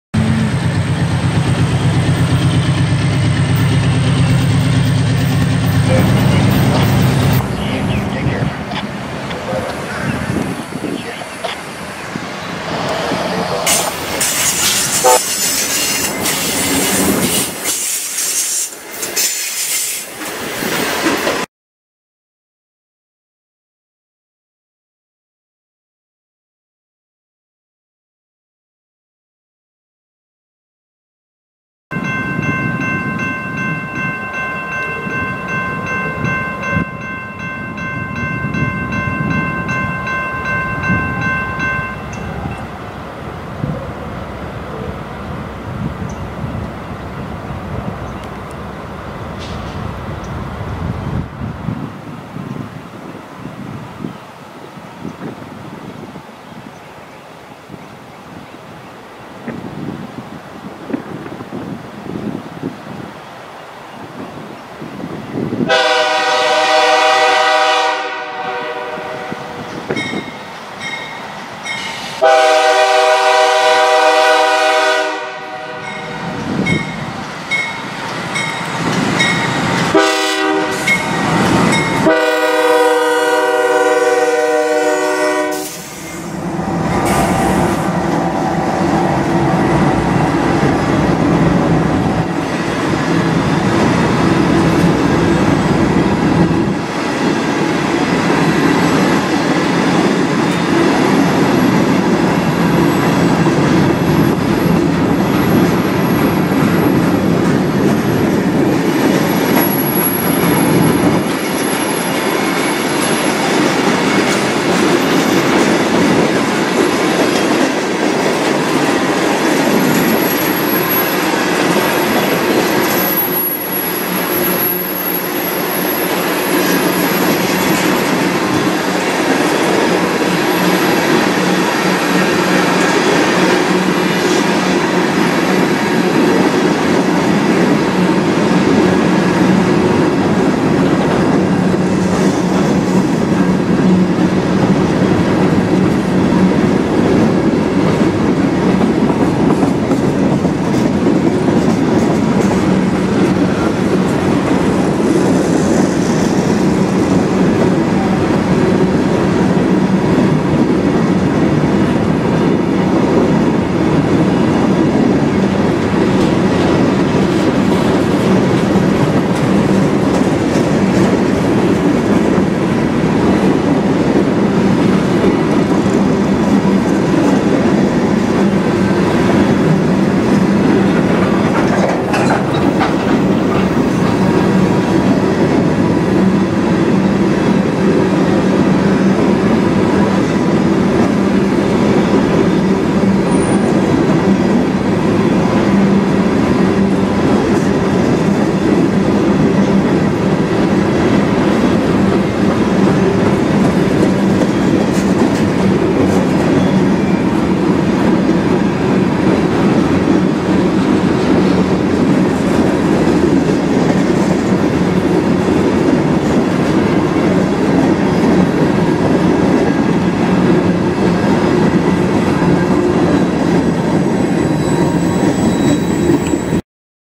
K311 Approaches Welcome Through The Woods
One of the best locations in the Bone Valley, in my opinion. Here is K311-29 approaching South Welcome on January 29 With the traditional GR-1 train in tow.